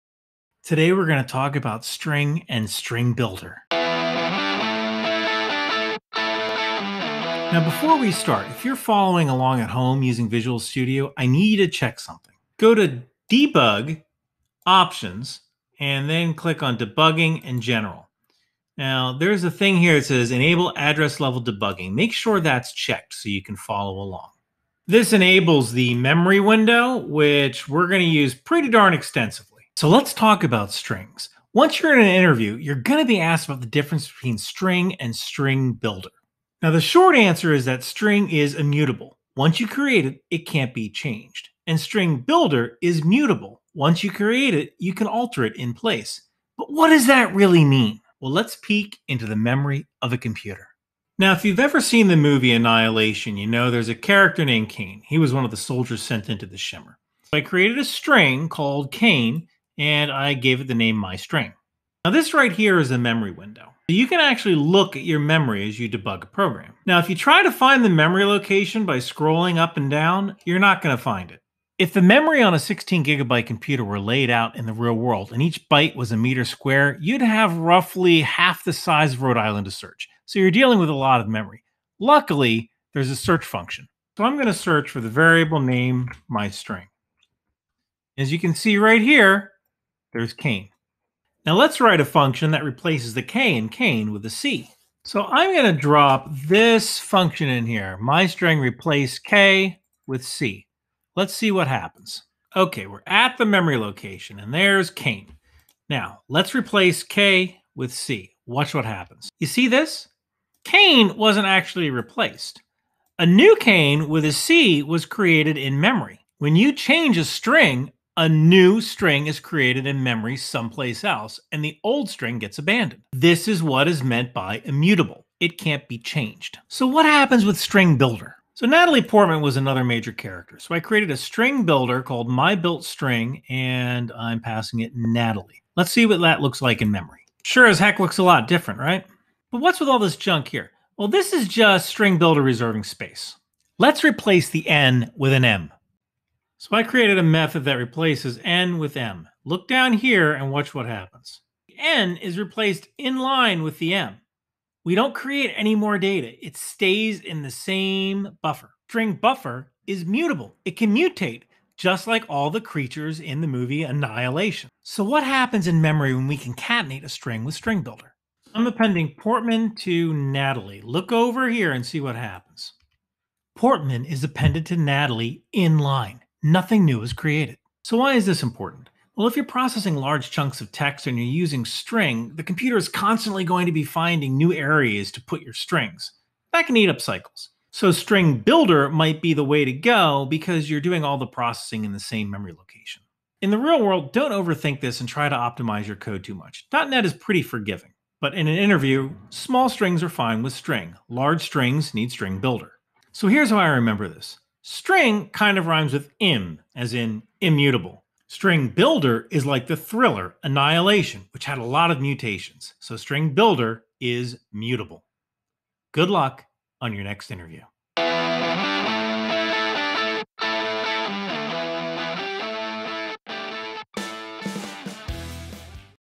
String and StringBuilder - C# Gotcha Interview Questions
What is the difference between String and StringBuilder in C# Dotnet? Today we look at what's actually going on in memory when you create a String or use StringBuilder.

Note:
If you are following along with Visual Studio, make sure you have "Enable address-level debugging" checked.  To do this, go to:
Debug } Options
Select "Debugging } General"
and make sure "Enable address-level debugging" is checked.